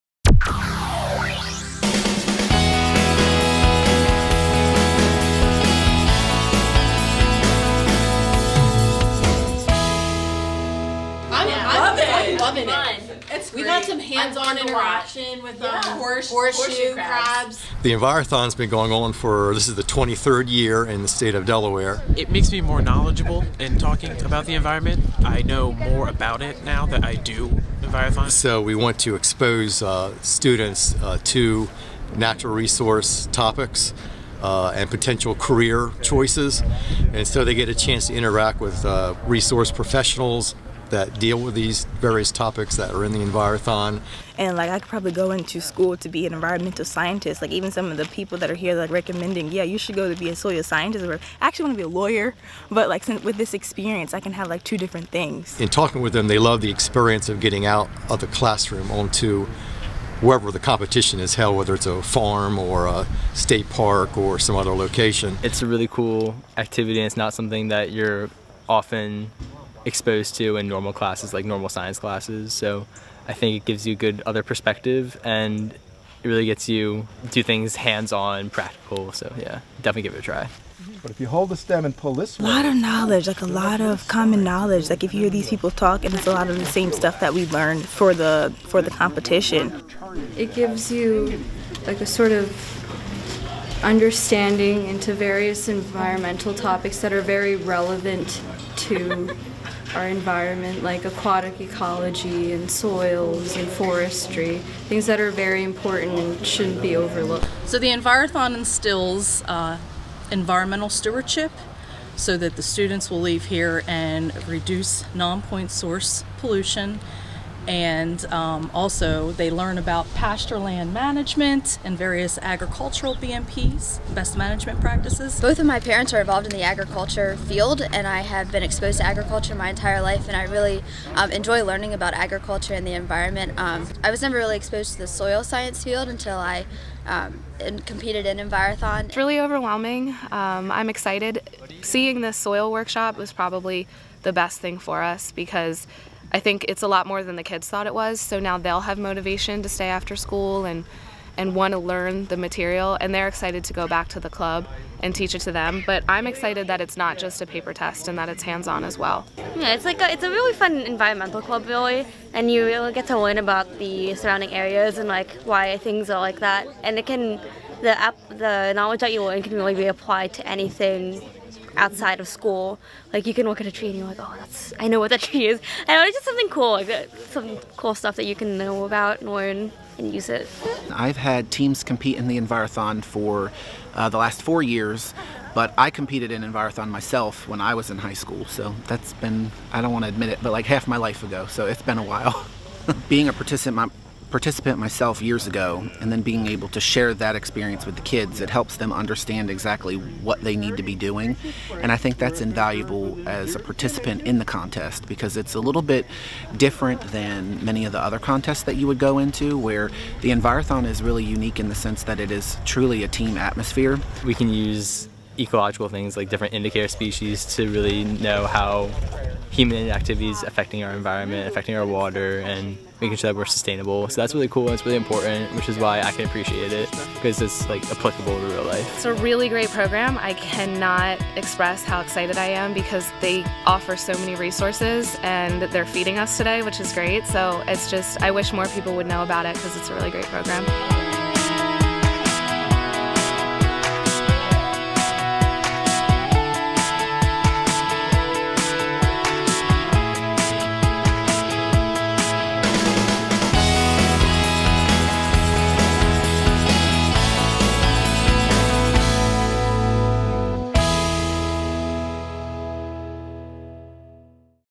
Envirothon
The Delaware Envirothon provides students with an integrated approach to exploring five natural resource categories. It tests their creativity, analytical thinking, and team-building skills in a competitive format. Envirothon is a "day-in-the-field" where teams visit testing stations for problem solving opportunities in Air Quality, Aquatic Ecology, Forestry, Public Speaking, Soil/Land-Use, Wildlife, and a Current Environmental Topic. The winning team represents Delaware at the National Conservation Foundation (NCF) Envirothon held each year in late July or early August.